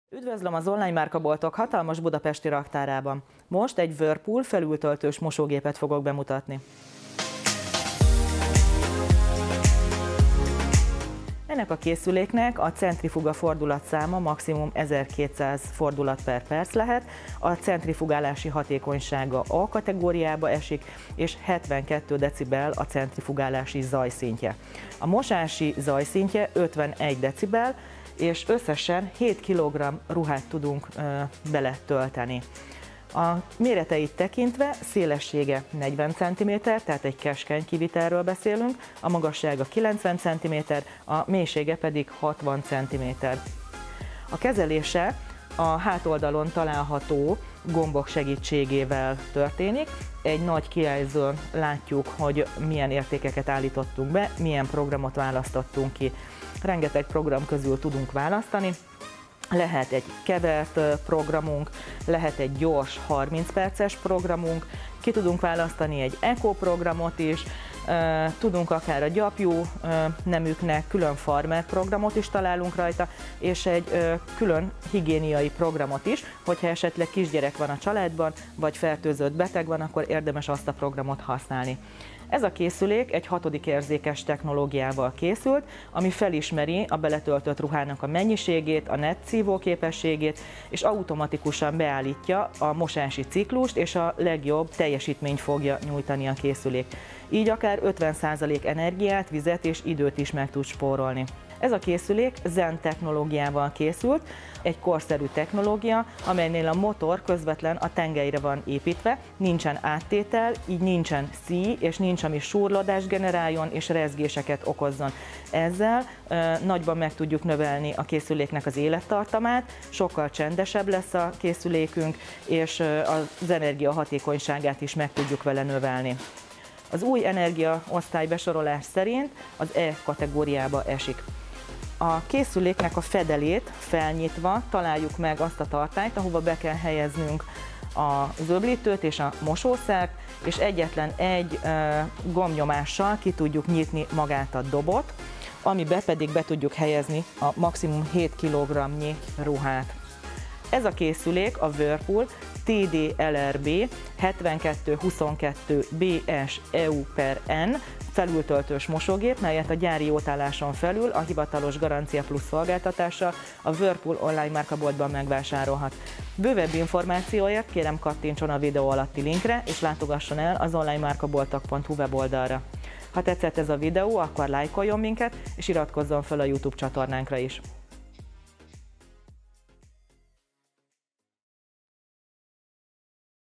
Whirlpool TDLRB 7222BS EU/N Felültöltős mosógép bemutató videó
A bemutató videóban szereplő Whirlpool TDLRB 7222BS EU/N Felültöltős mosógép 5 év garanciával megvásárolható a Whirlpool onlineMárkaboltban. További információkért kattintson a következő linkre:

A Whirlpool TDLRB 7222BS EU/N Felültöltős mosógép leghasznosabb tulajdonságai
►Energiaosztály  E
►Centrifuga fordulatszám  1200 fordulat / perc
►Mosási töltetsúly  7 kg
►Inverter motor